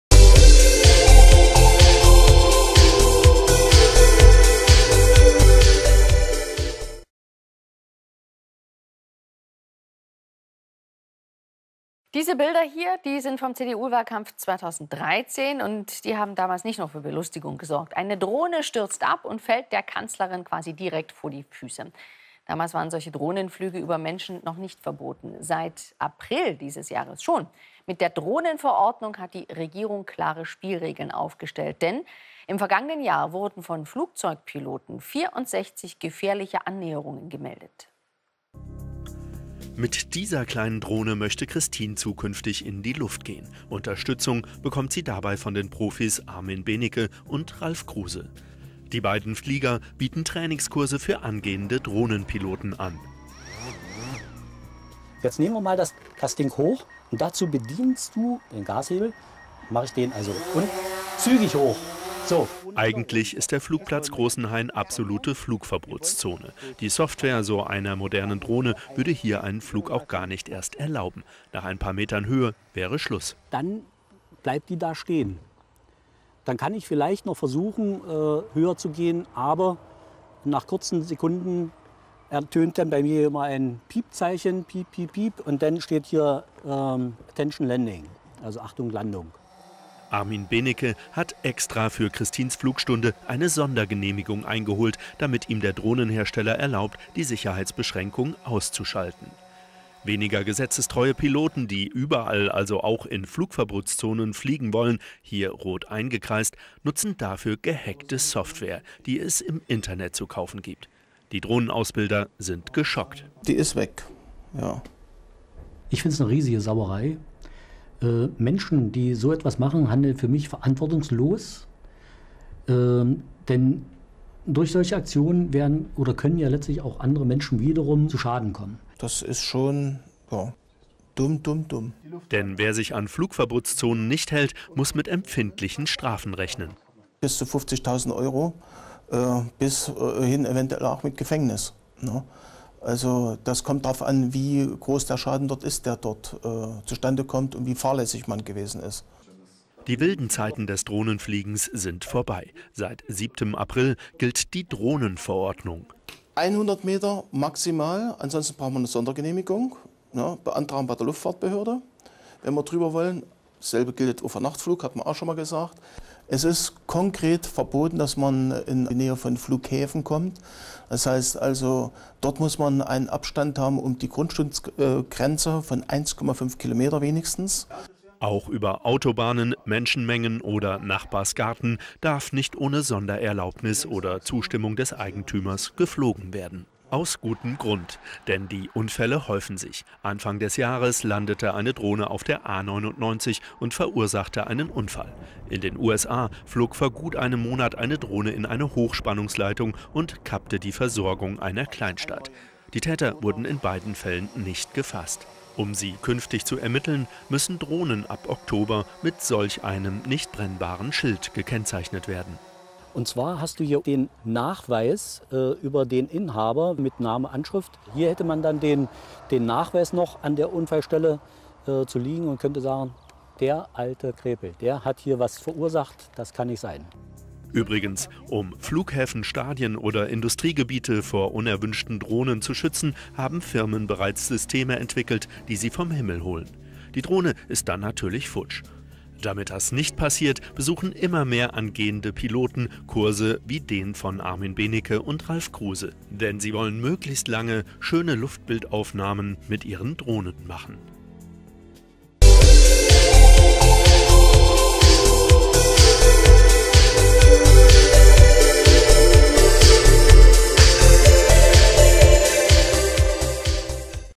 Fernsehbeitrag vom Falko Schuster - Nutzung von UAS (Drohnen) 13.07.2017 - by Armin Benicke
Fernsehbeitrag vom Falko Schuster und Team - Sichere Nutzung von UAS (Drohnen) mit Ralf Kruse vom Fliegerservice August der Starke und dem Versicherungsfachmann (BWV), Pilot und Drohnensteuerer Armin Benicke vom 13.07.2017. copyright by Armin Benicke, Pilot und Registrierter SafeDrone Pilot by Lufthansa Technik. Registrierungsnummer: P-2A3-G70. Generalagentur der Continentale Versicherung a.G.